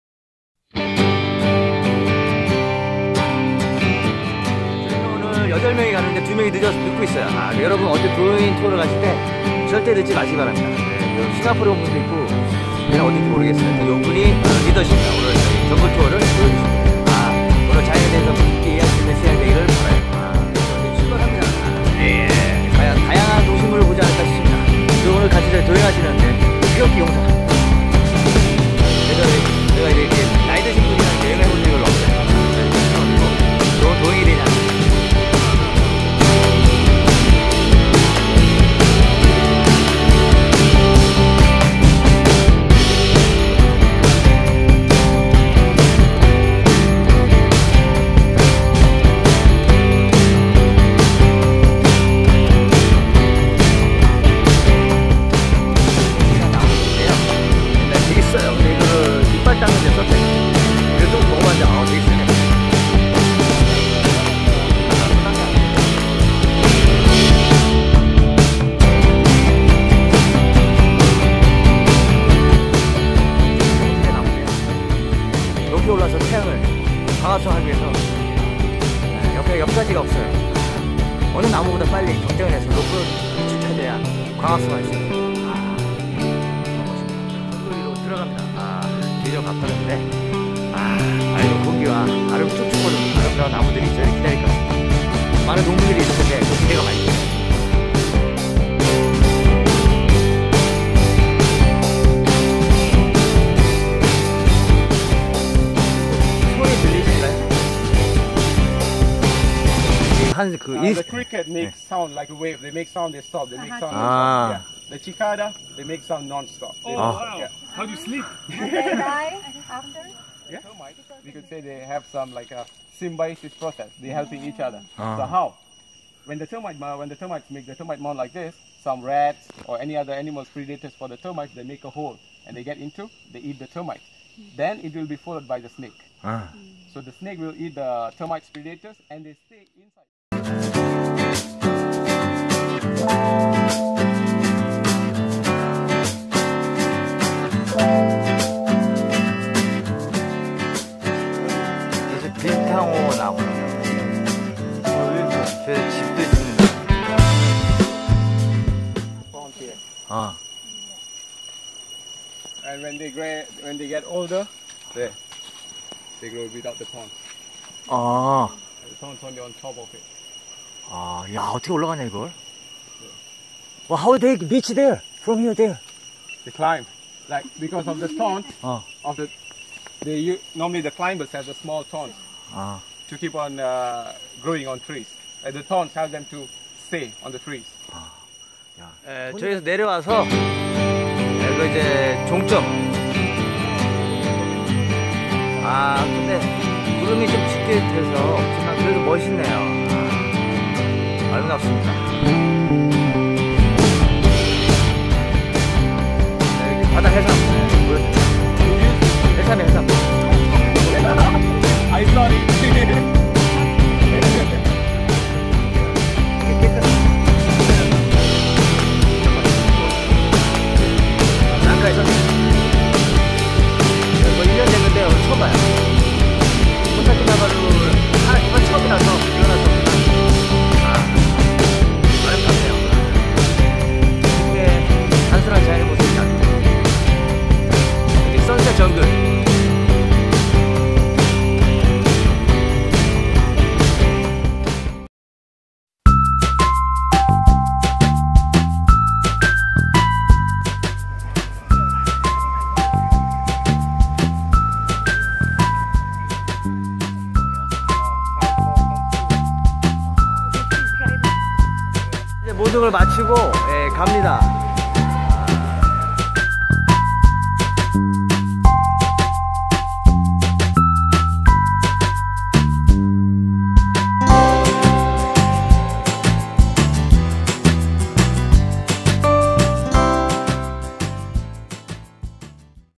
랑카위 여행 투어, 정글도,바다도 선쎗도 즐기는 랑카위 정글 투어, 힐링최고, Langkawi Jungle Trecking
유네스코 보존 지역의 밀림을 투어하는 랑카위 정글 트래킹 영상 입니다. 아름다운 정글 속에서 자연과 맑은 공기를 즐기는 랑카위 정글 트래킹 투어는 가족분과 연인들에게 좋은 거 같습니다.